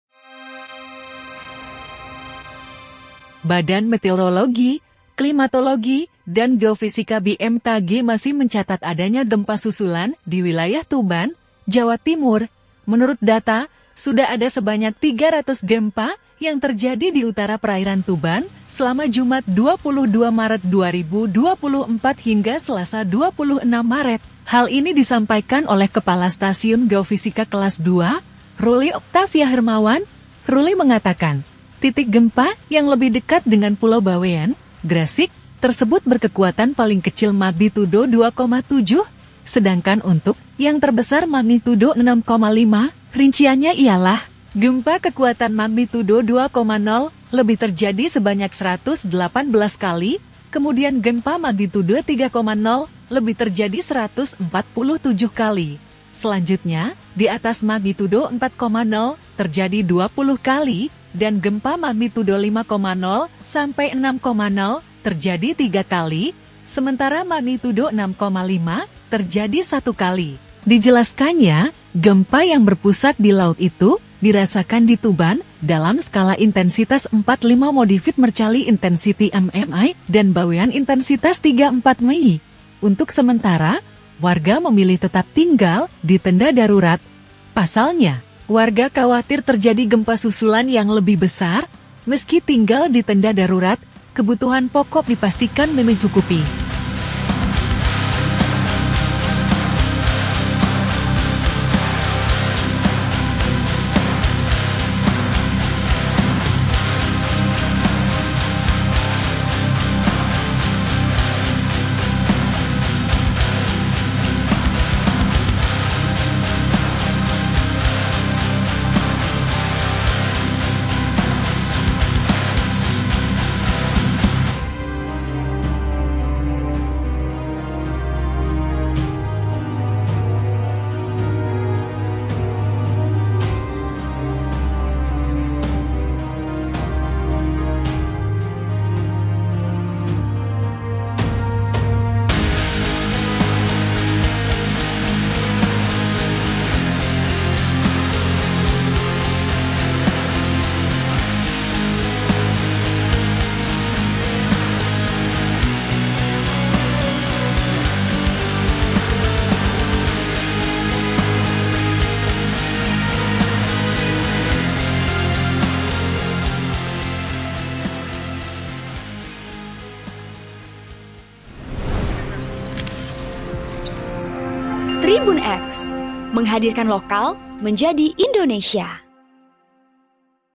⚪ UPDATE❗ Tuban Masih Dilanda Gempa Susulan, BMKG Catat 300 Kali Gempa Selama 4 Hari
Badan Meteorologi, Klimatologi dan Geofisika (BMKG) masih mencatat adanya gempa susulan di wilayah Tuban, Jawa Timur.

Menurut data, sudah ada sebanyak 300 gempa yang terjadi di utara perairan Tuban selama Jumat (22/3/2024) hingga Selasa (26/3).

Hal ini disampaikan oleh Kepala Stasiun Geofisika Kelas II, Rully Oktavia Hermawan.

Rully mengatakan, titik gempa yang lebih dekat dengan Pulau Bawean, Gresik, tersebut berkekuatan paling kecil magnitudo 2,7.

Sedangkan untuk yang terbesar magnitudo 6,5.

Rinciannya ialah, gempa kekuatan magnitudo 2,0 lebih terjadi sebanyak 118 kali.

Kemudian gempa magnitudo 3,0 lebih terjadi 147 kali.

Selanjutnya, di atas magnitudo 4,0 terjadi 20 kali, dan gempa magnitudo 5,0 sampai 6,0 terjadi tiga kali.

Sementara magnitudo 6,5 terjadi satu kali.

Dijelaskannya, gempa yang berpusat di laut itu dirasakan di Tuban dalam skala intensitas IV-V Modified Mercalli Intensity (MMI) dan Bawean intensitas III-IV MMI.

Untuk sementara, warga memilih tetap tinggal di tenda darurat.

Pasalnya, warga khawatir terjadi gempa susulan yang lebih besar.

Meski tinggal di tenda darurat, kebutuhan pokok dipastikan mencukupi.